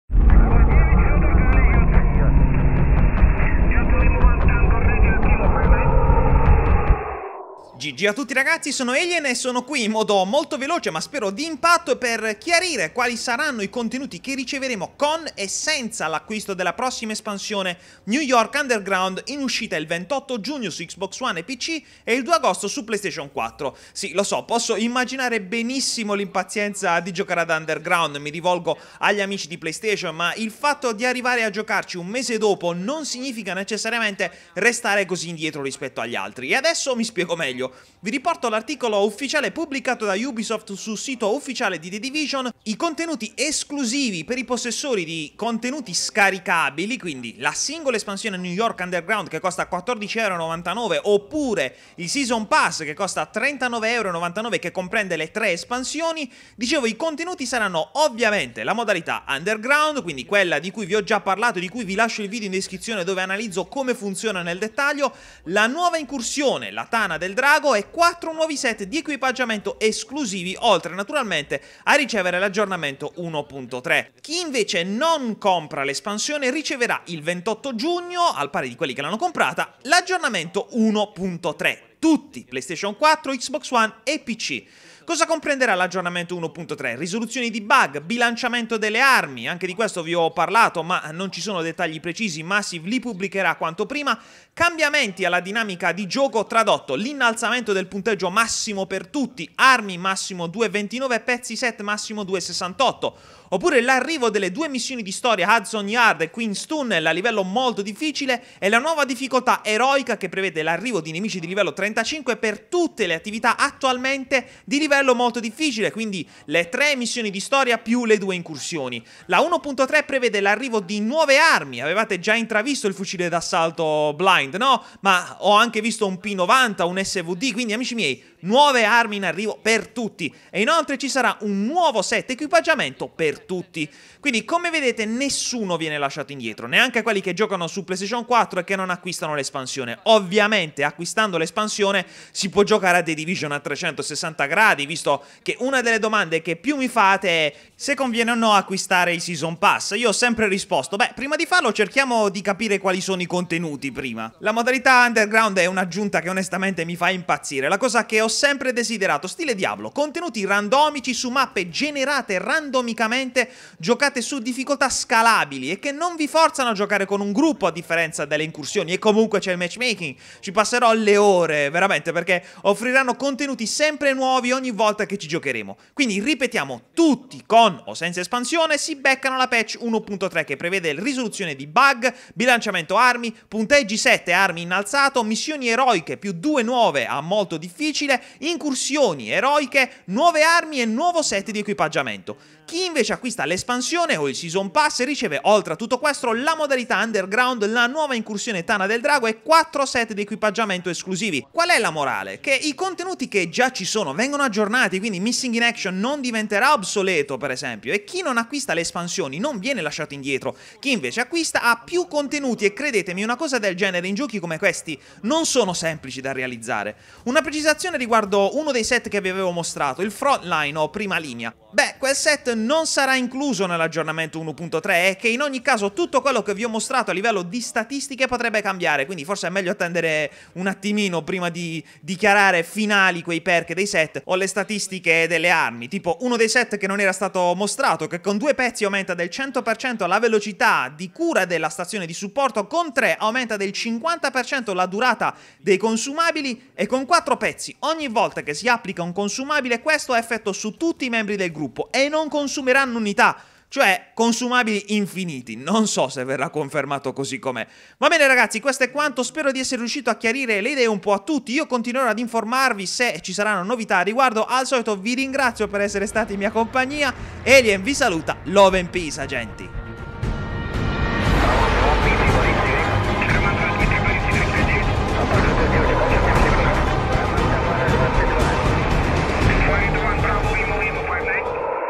The Division: UNDERGROUND - Cosa si ottiene CON e SENZA ESPANSIONE (su TUTTE le Piattaforme!)
Una delle domande se non la principale domanda che ci stiamo ponendo: cosa otterremo acquistando l'espansione New York Underground e quali saranno le differenze rispetto a chi non acquista? Ci saranno svantaggi per chi non acquisterà? Spaccherà la community ? Massive ha risposto a tutte queste domande.

5% di SCONTO su GAMEXP.IT: usa il codice ALIENXP (IN MAIUSCOLO)